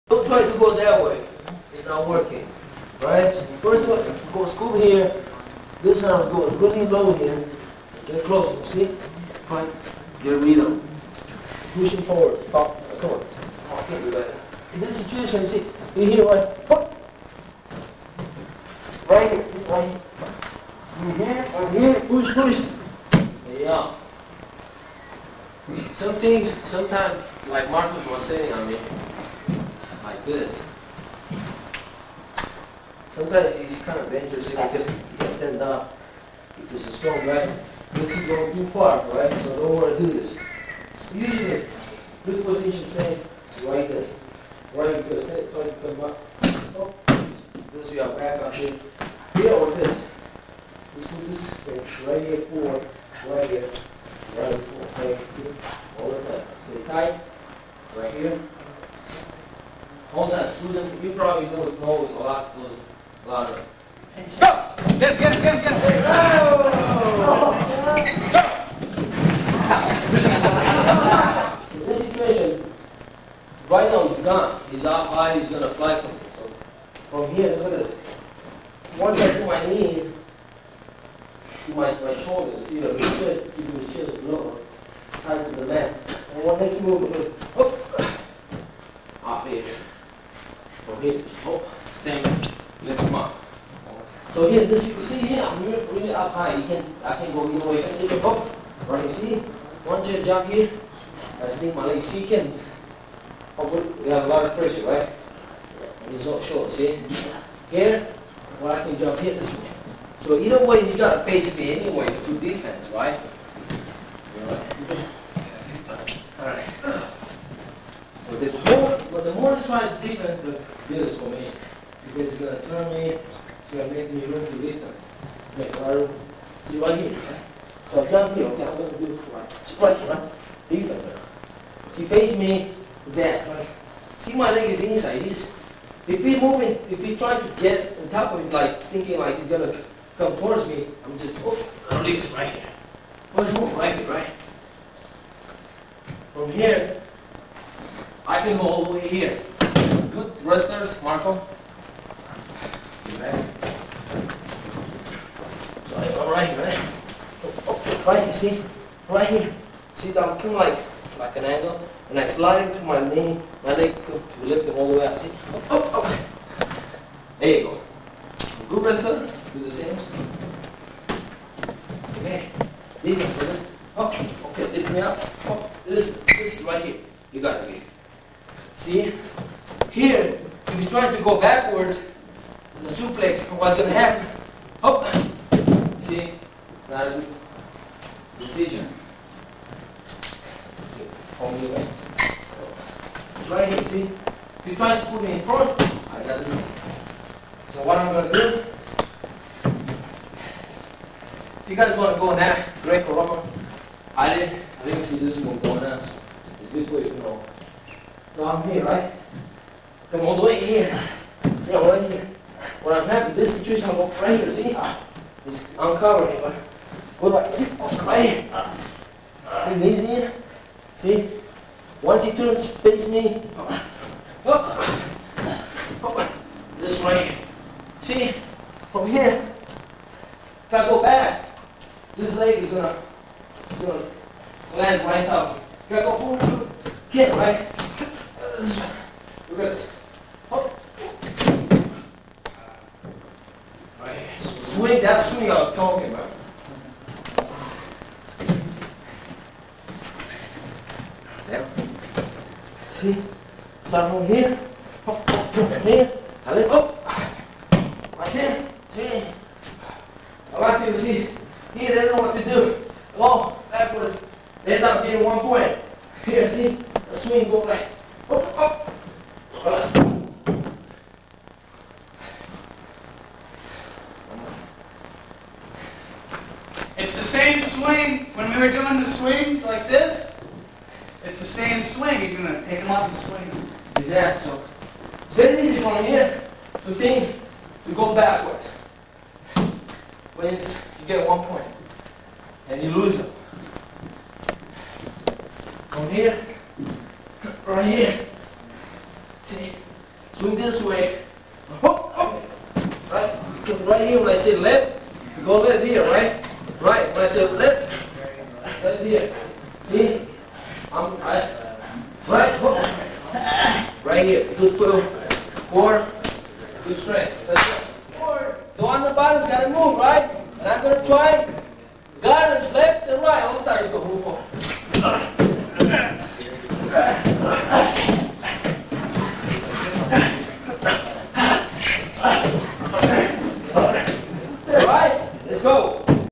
World Champion Leo Frincu Greco Wrestling Moves
World Champion Leo Frincu Wrestling Seminar